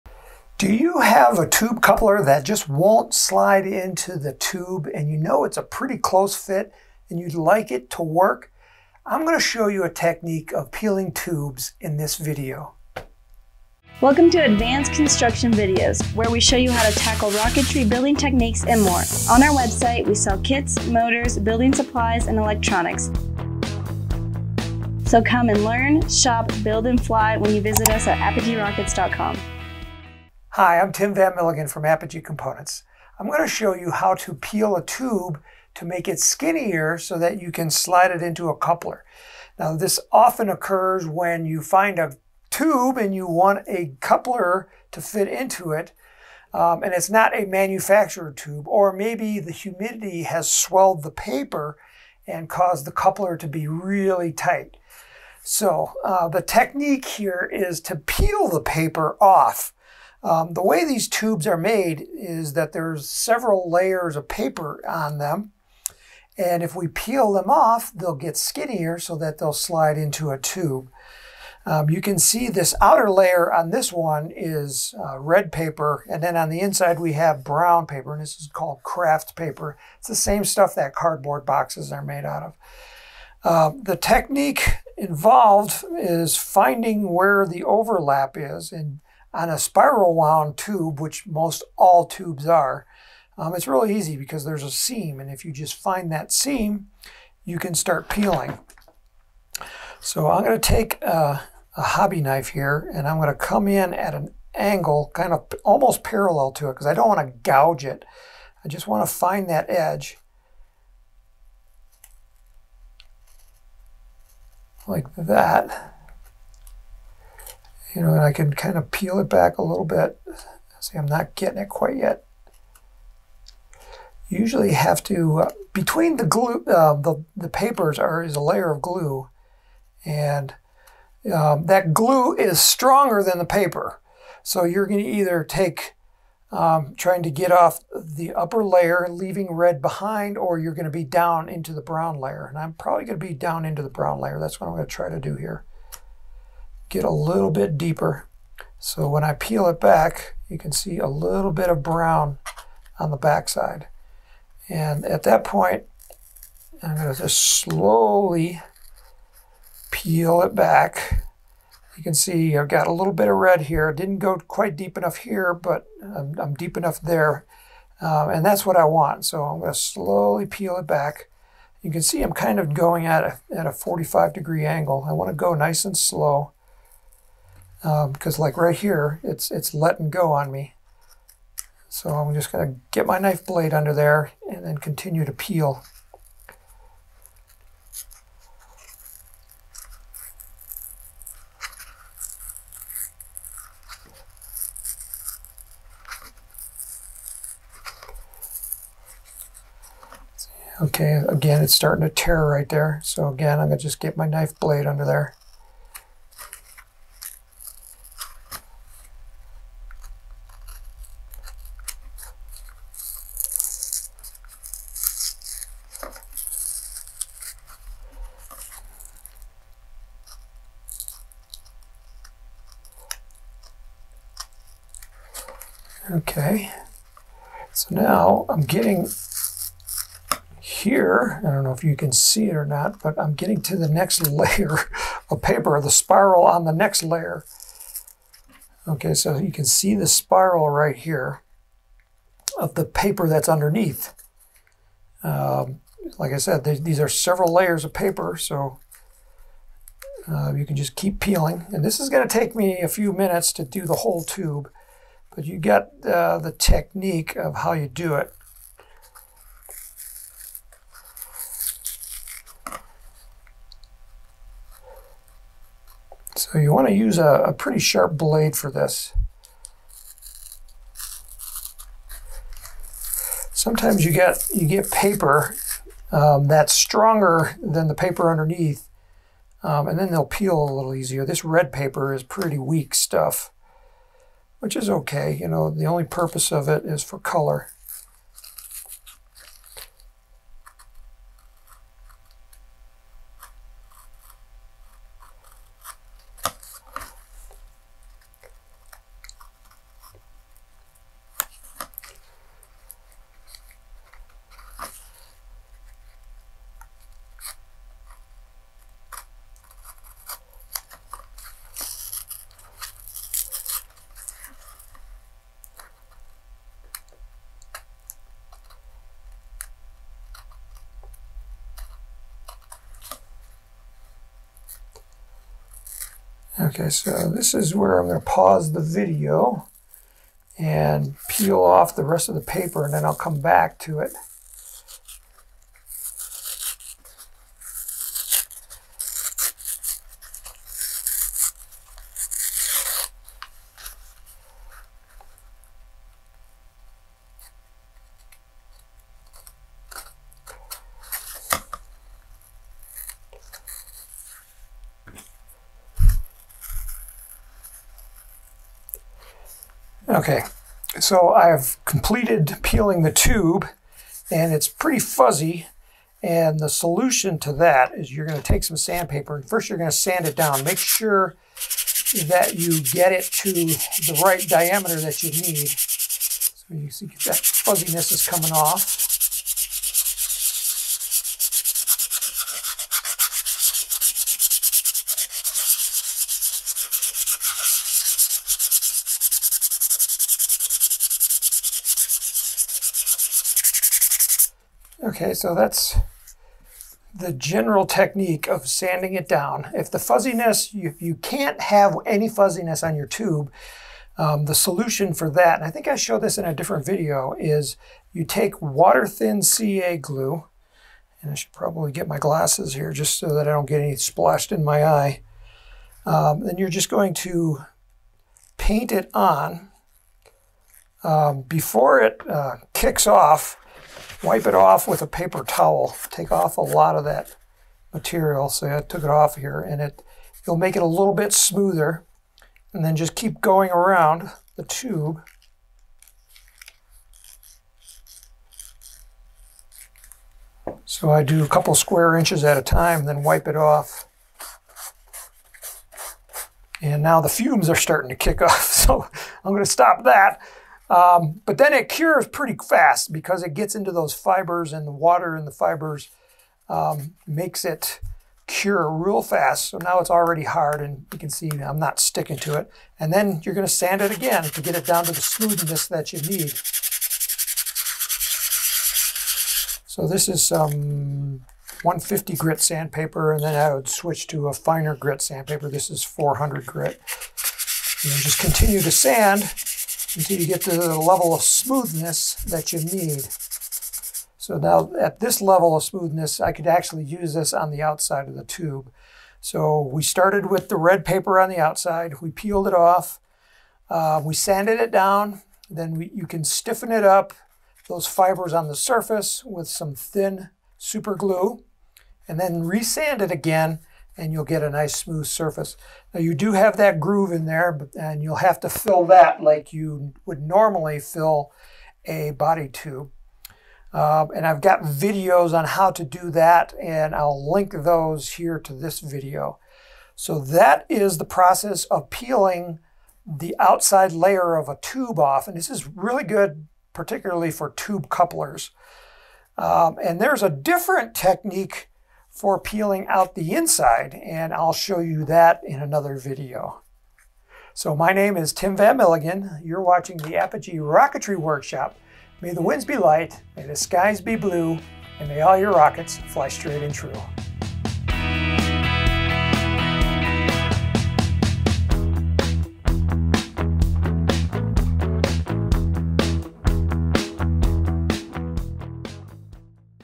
Peeling a paper coupler for model rockets
- Do you ever find a tube coupler that is too tight to fit into the tube? The solution to his problem is to peel a layer of paper off the outer surface of the coupler. It can be a little tricky because they are usually glued well.

In this video, I'll show you my technique for peeling a layer of paper off the surface using nothing more than a hobby knife. And then you'll learn how to get rid of the fuzziness that will remain. Removing the fuzziness is optional, as most time you won't need to do it. But you'll see how to get them super smooth if that is your desire.